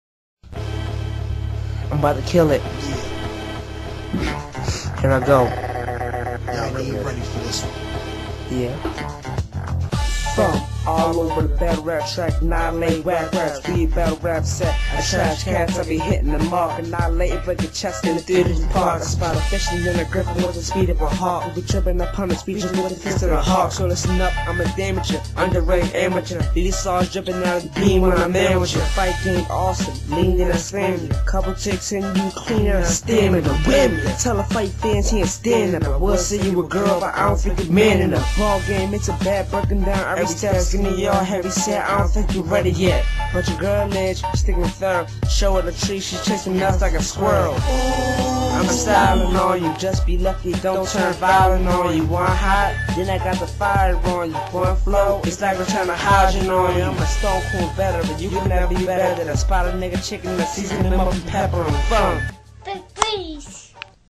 FUM SPAZING
F.U.M THE YOUNG LEADER OF THE BABY BEANS TEAM GOT SOME BARS OF HIS OWN TO GET OFF HIS CHEST CHECK HIM OUT AS HE DO HIS THING